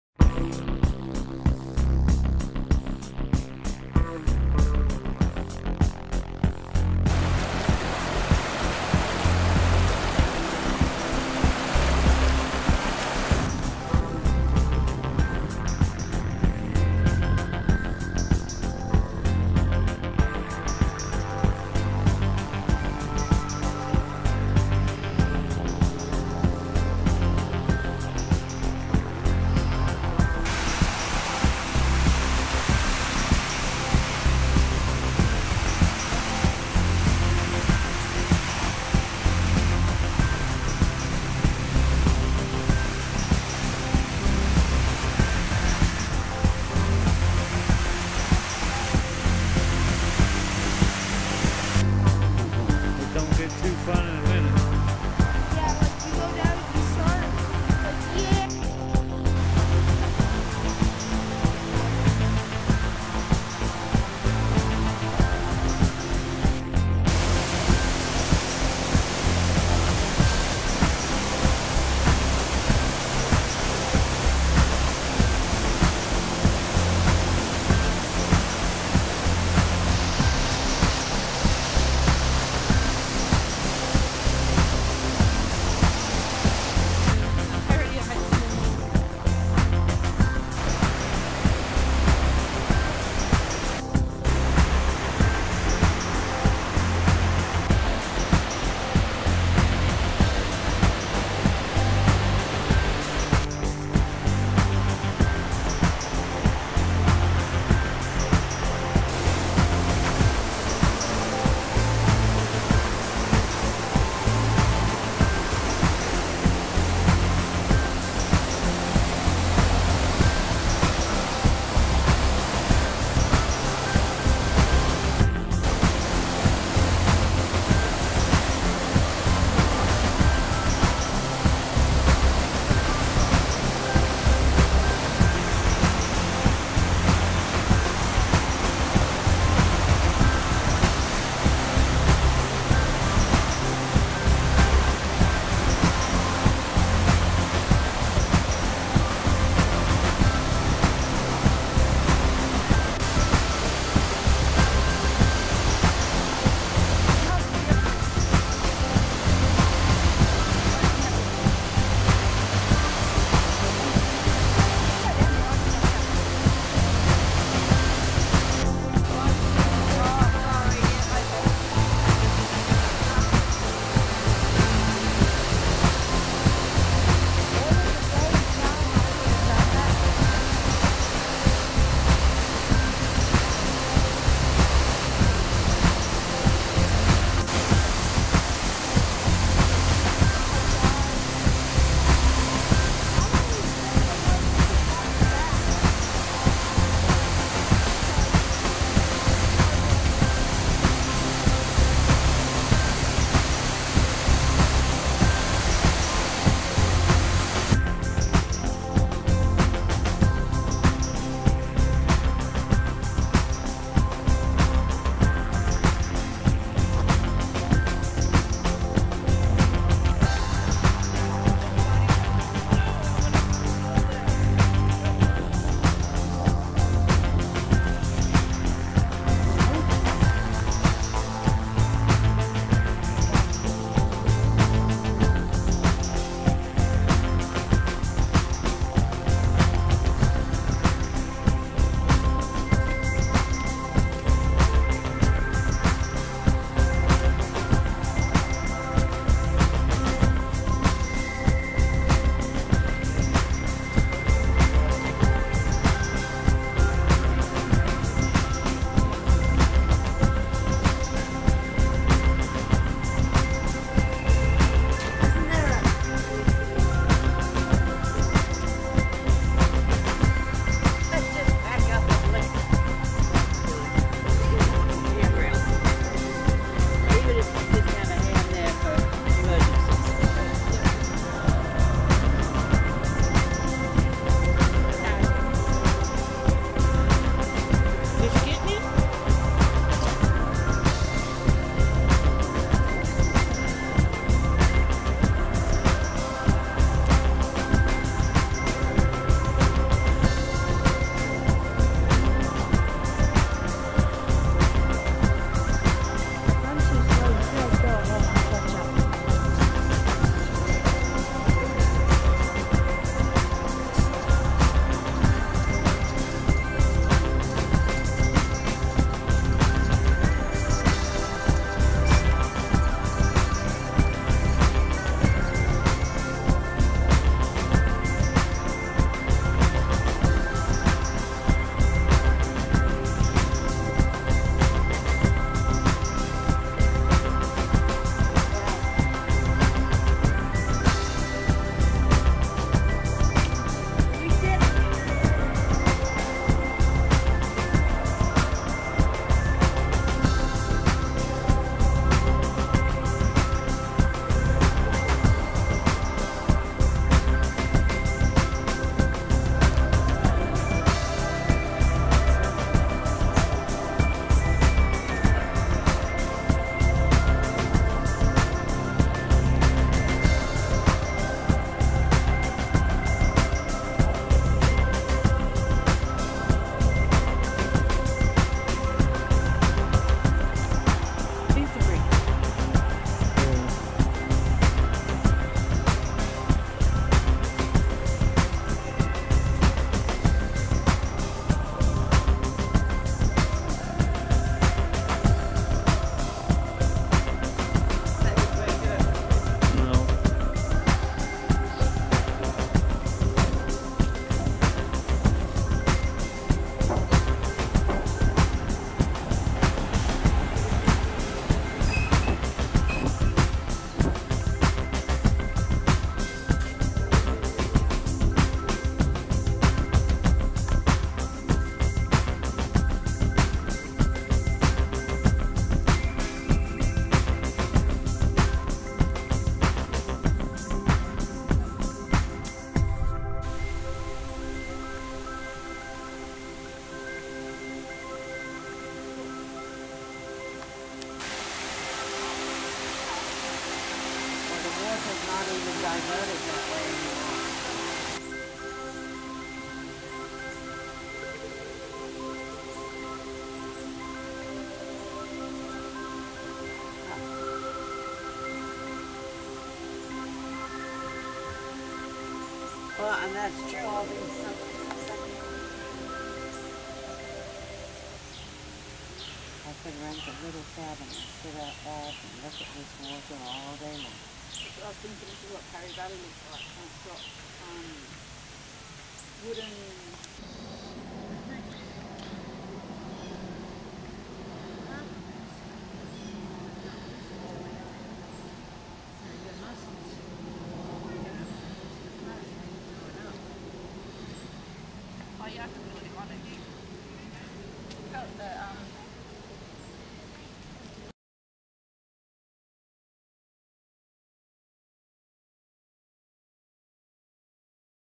Never Alone.
This is a video from the weekend I spent with my Sisters, one I had never met before and came from Australia to visit! Special thanks to my Friend Hypnos Rêveur for the use of the song which is so fitting for this video.